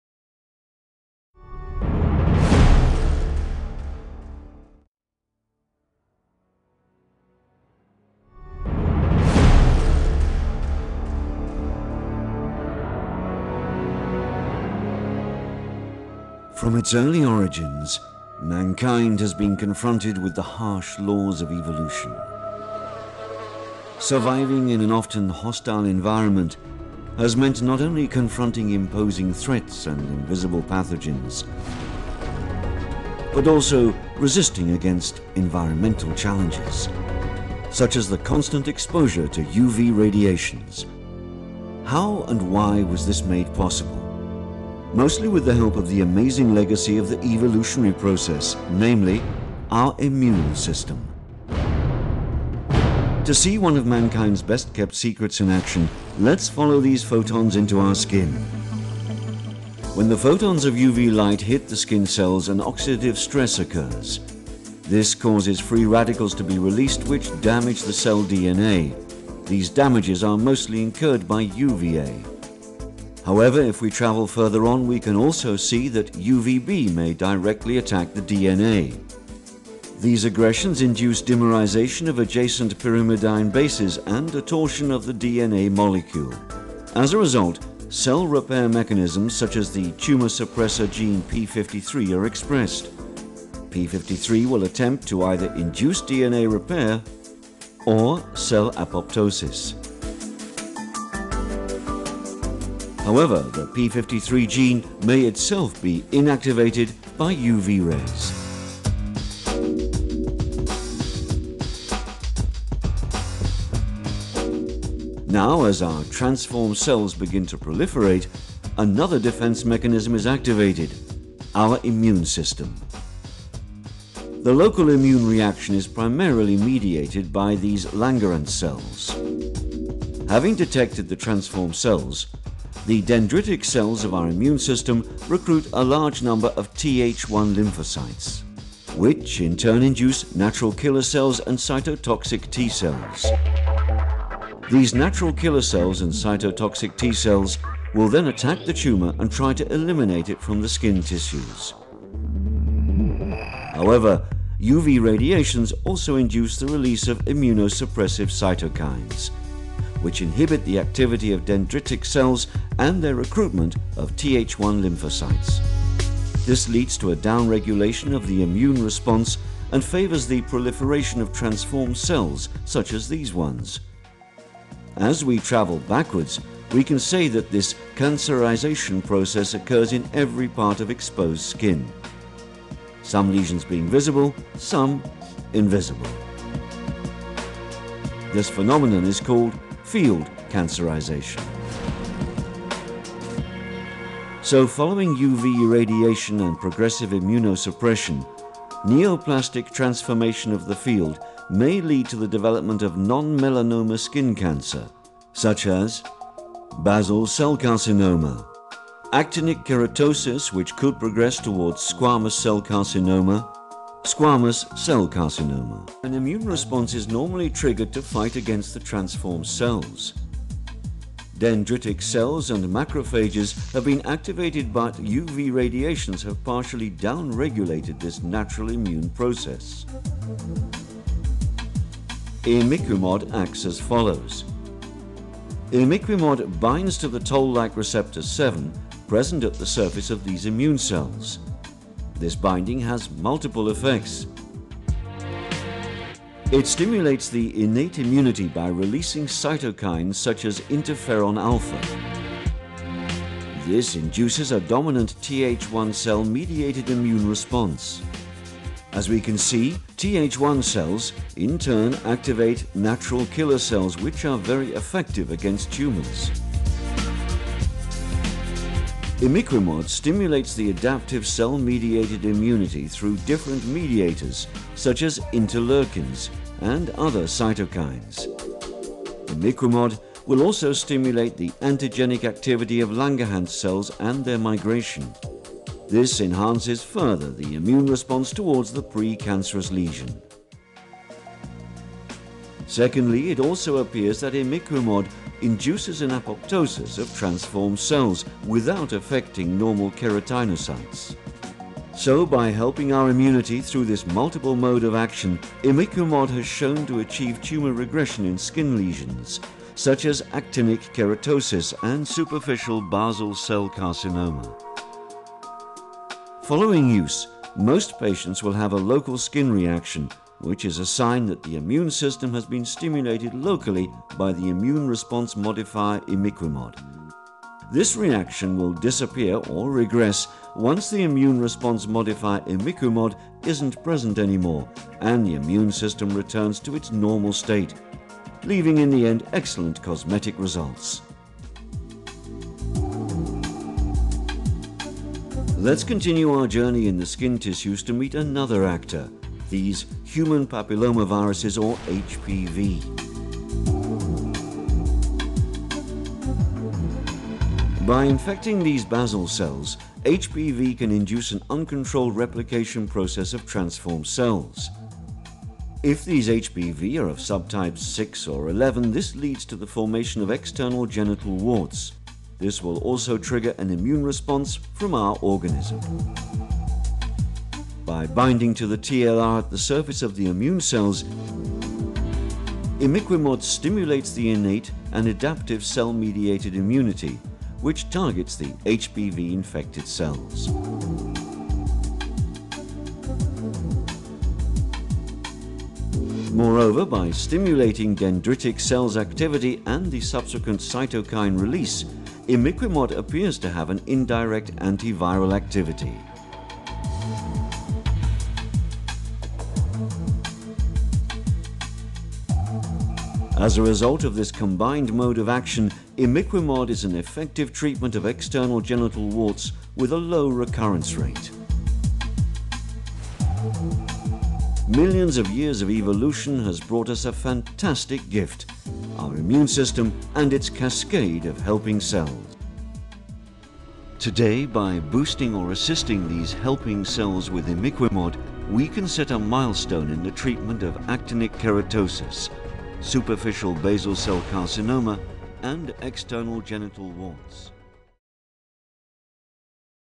Utazás a bőrben
Az ősidőktől kezdve az emberiség ki volt téve az evolúció kemény törvényeinek. Túlélni egy gyakran barátságtalan környezetben nem csak azt jelentette, hogy szembe kellett szállni a felmerülő fenyegetettséggel és a látható veszélyekkel, hanem azt is, hogy ellen kellett állni a környezet kihívásainak is, például a folyamatos UV-sugárzásnak. Hogyan és miért volt ez lehetséges? Főleg az evolúciós folyamat lenyűgöző örökségének segítségével, nevezetesen az immunrendszernek köszönhetően. Hogy láthassuk az emberiség egyik legféltveőrzöttebb titkát működés közben, kövessük ezeket a fotonokat a bőrünkbe...